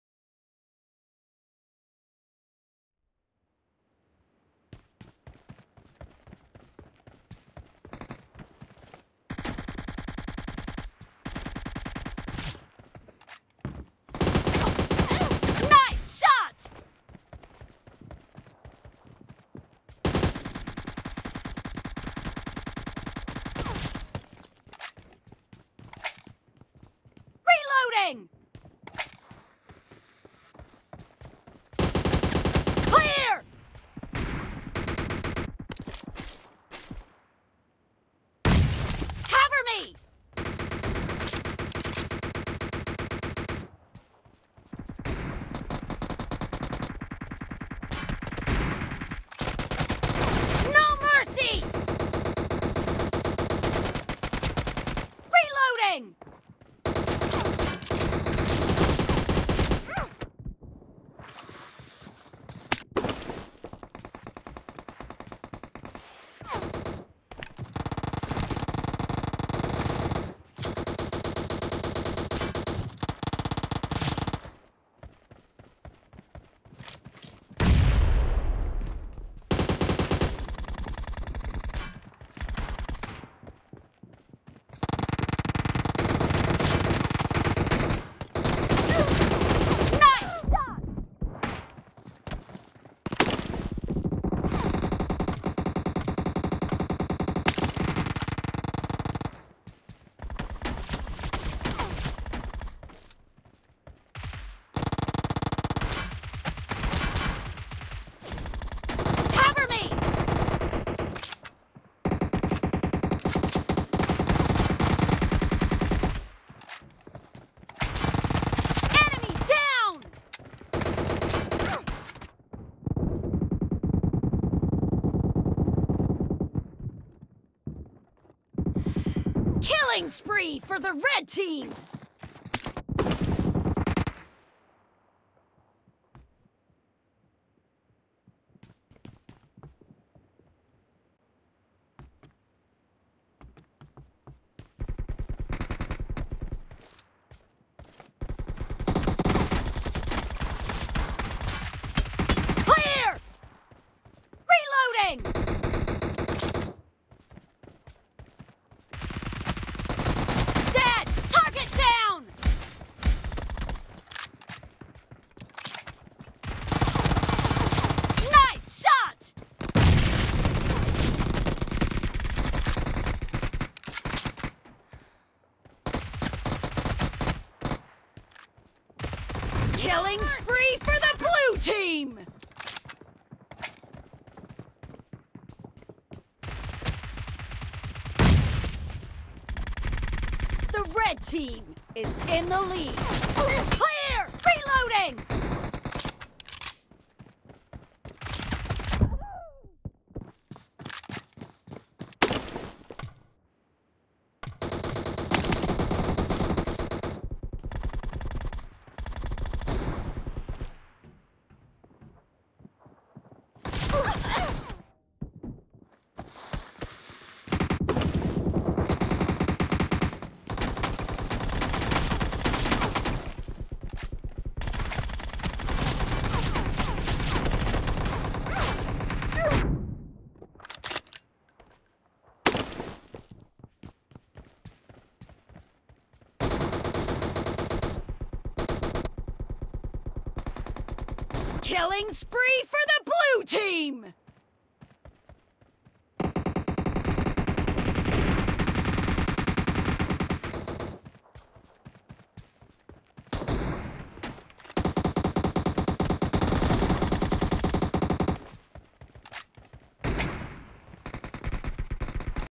FPP TDM Practice after new 4 finger layout
Hey guys it's my first video on YouTube with my new 4 finger layout